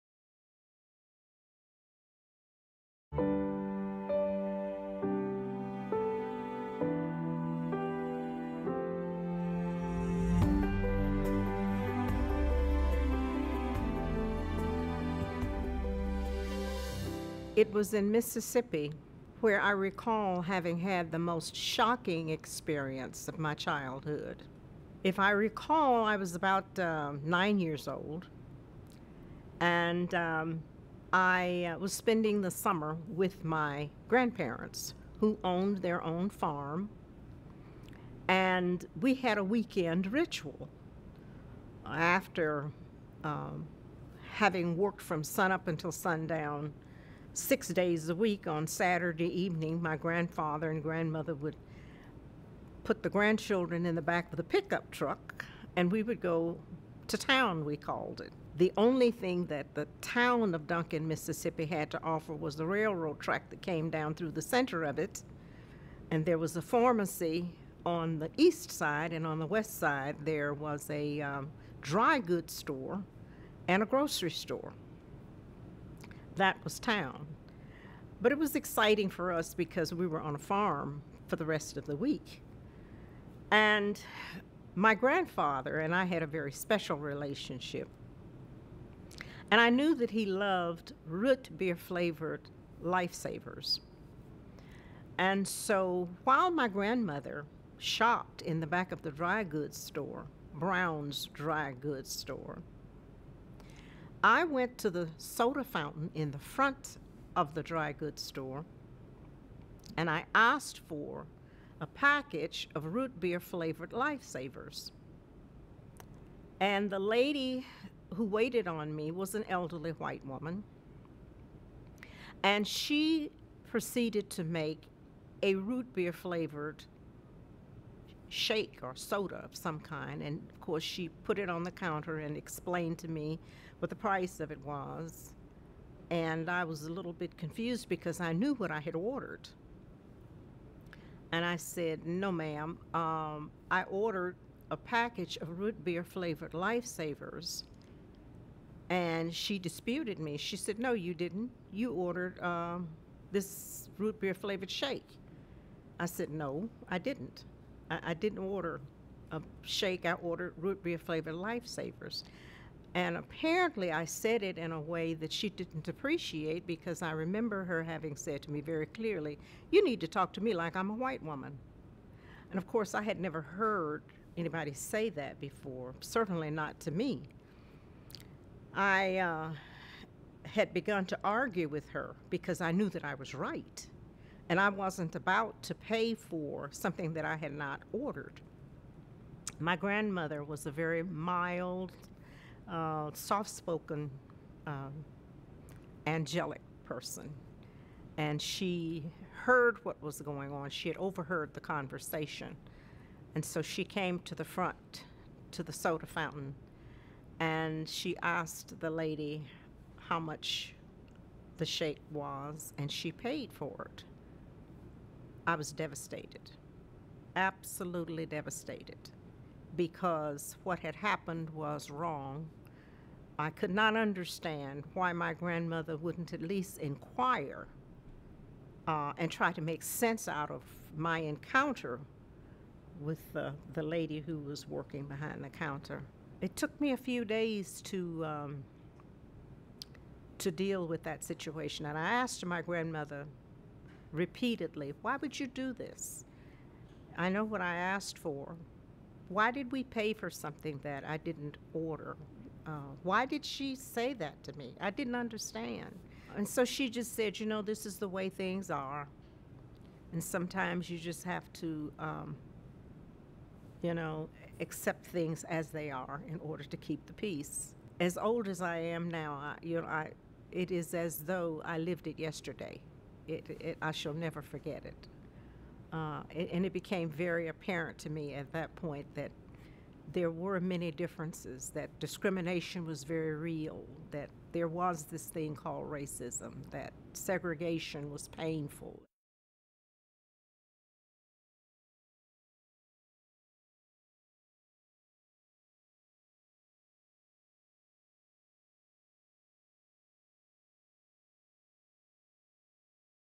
Deanie Parker, Ret. President, Soulsville Foundation Shares A Story of Discrimination
Deanie Parker, former Stax Recording Artist and Retired President of the Soulsville foundation shares an early account of racial discrimination from her childhood in Mississippi.

Browse through more than 100 interviews or add to the collection by sharing your own story or that of a loved one.